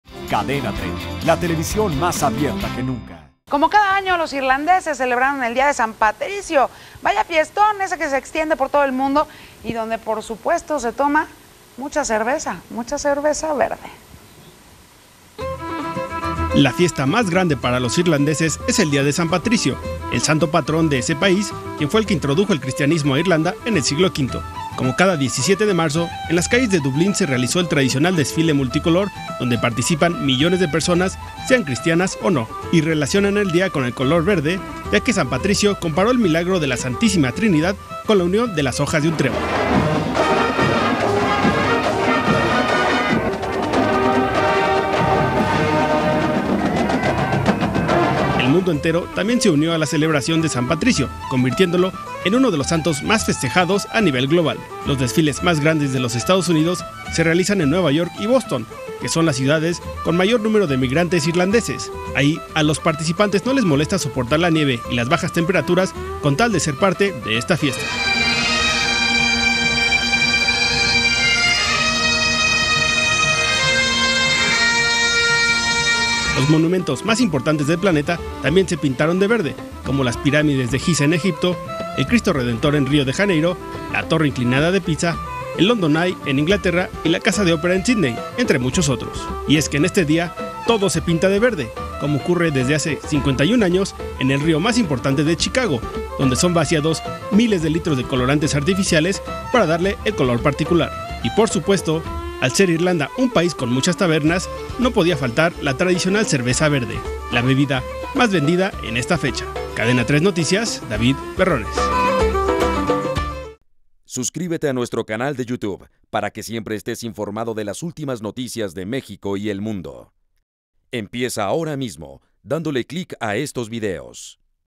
Celebra Irlanda y el mundo el Día de San Patricio; monumentos se pintan de verde
Desde la Quinta Avenida de Nueva York y las zonas pantanosas de Luisiana hasta la Plaza Parnell de Dublín, miles de personas se tiñeron de verde para celebrar el Día de San Patricio, la fiesta más importate de Irlanda.